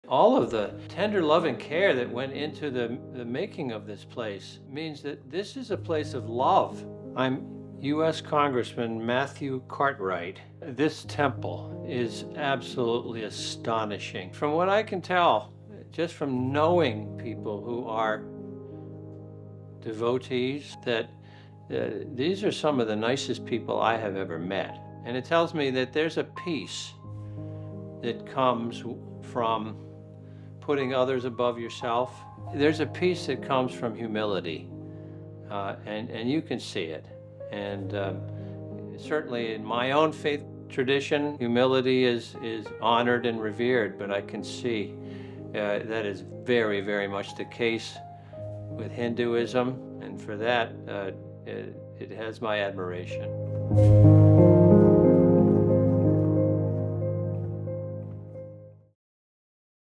US Rep Matt Cartwright (PA-8) shares his admiration for Akshardham
Astonishing in its beauty and inspiring in its values. Congressman Matthew Cartwright shares his admiration for Akshardham, a place of love, humility, and putting others above oneself.